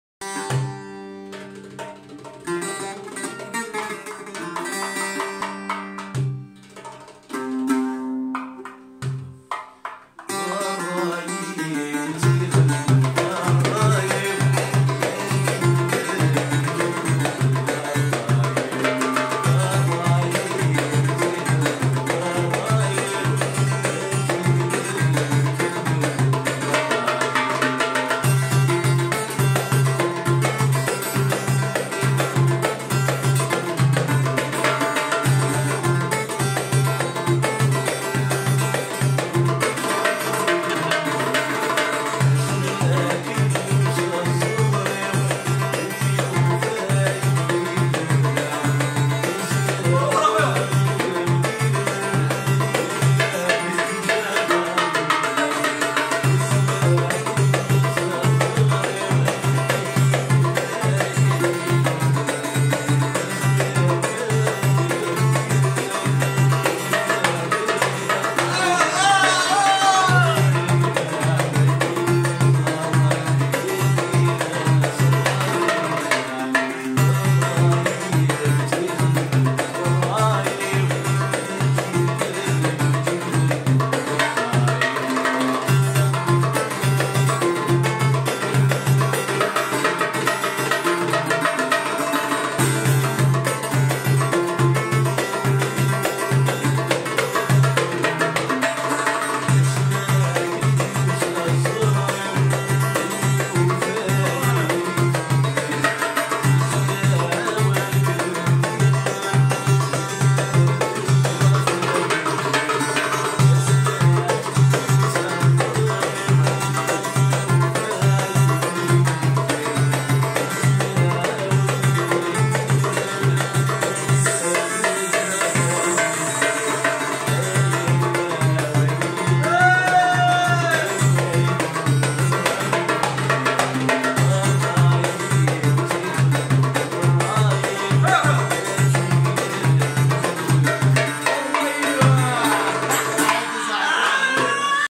Hocine yahiatene amar babaci à laanasar boghni mars 2018
Petite vidéo très sympa filmé par Mlle Rosa pendent le séjour très court en Algérie à boghni exactement.